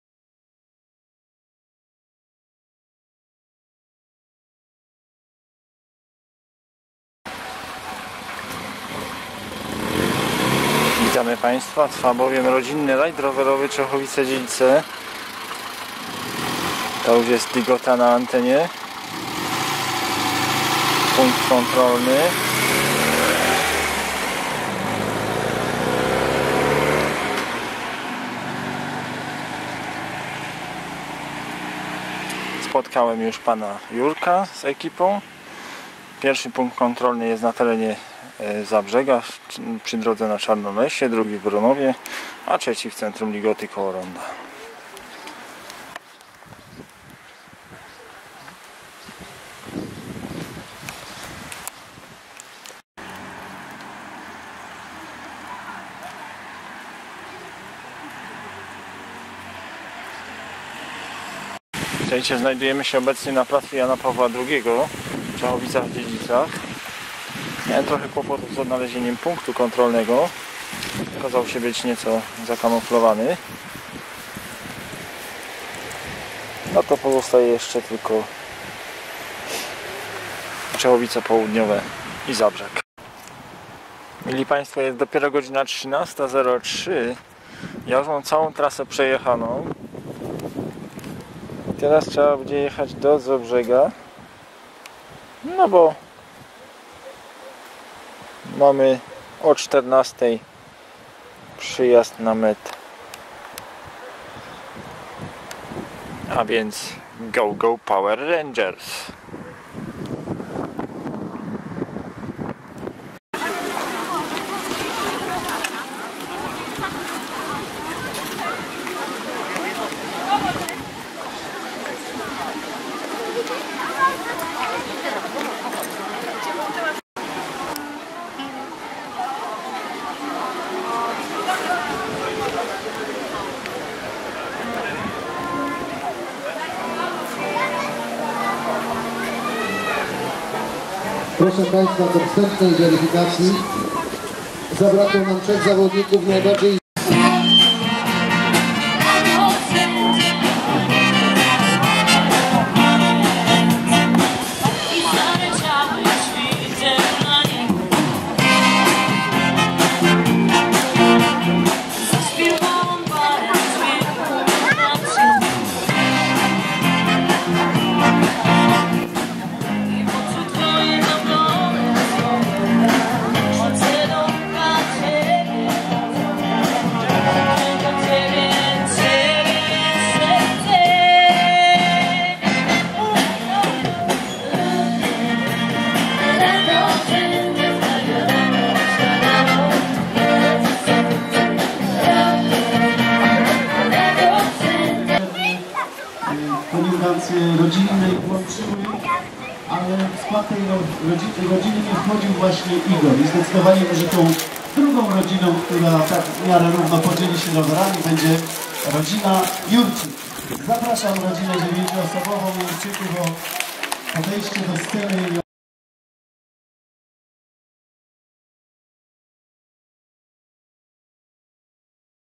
Rajd Rowerowy Czechowice Dziedzice 6 09 2015
Rodzinny Rajd Rowerowy Czechowic-Dziedzic, który odbył się 6 września 2015 roku.